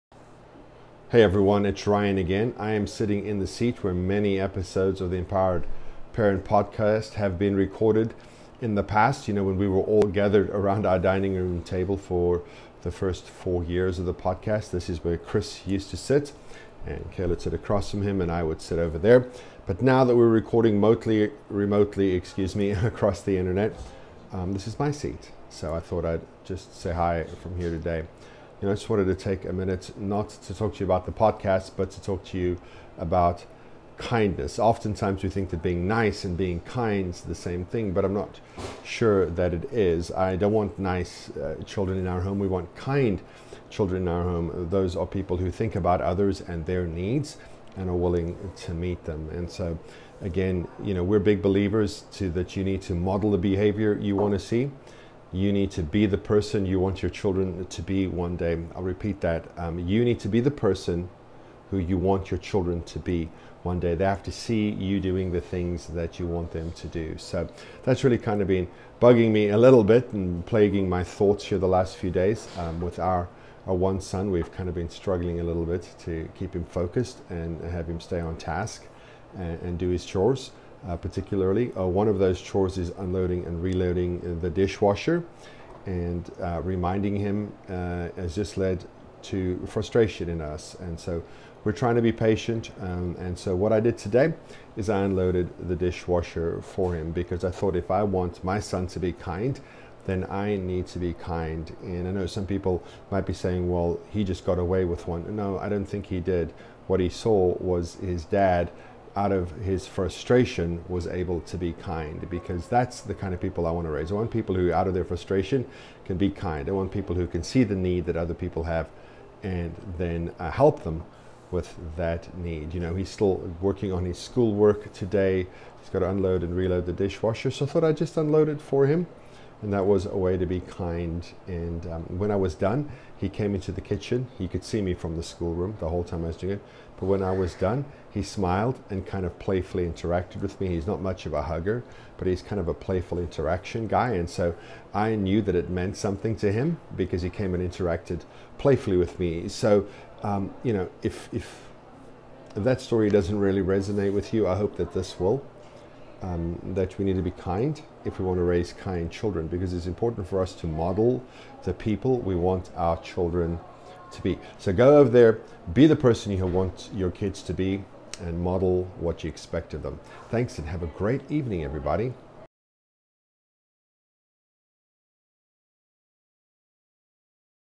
Why Being Kind Matters
Ryan talks about the importance of modeling kindness for our children.